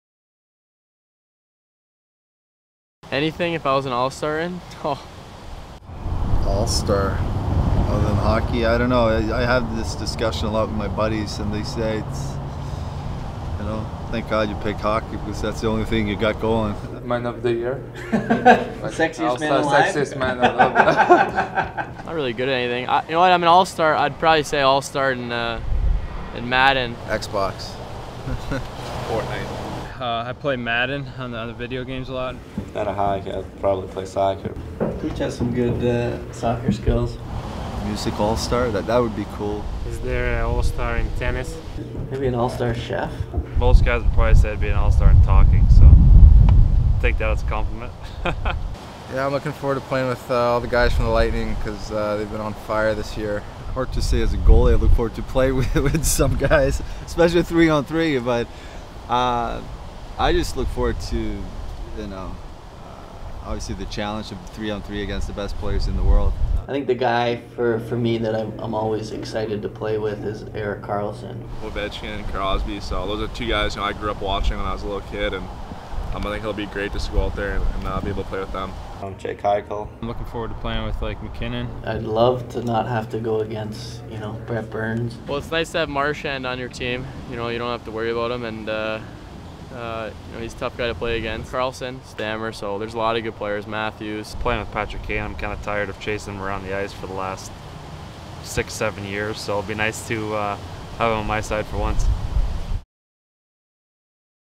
All-Star Weekend 2018 in Tampa
We caught up with a few of our All-Star athletes at NHLAllStar weekend in Tampa! Hear what they had to say.